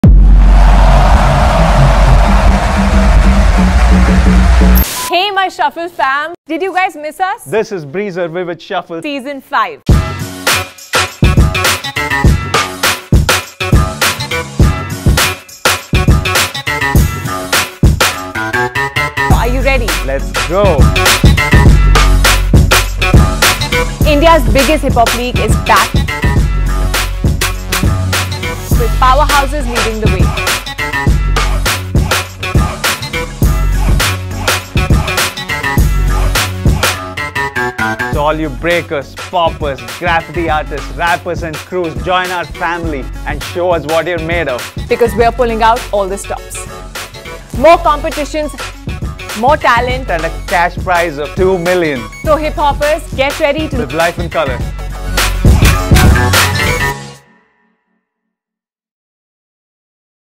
BREEZER Vivid Shuffle Season 5 is back!
This is BREEZERVividShuffle Season 5 - India's biggest hip-hop league. 🔥
We're set to LiveLifeInColour and bring home the VoiceOfTheStreets. You with us?😎

REGISTRATIONS OPEN - 20 AUGUST at 10 PM on breezervividshuffle.in and insider.in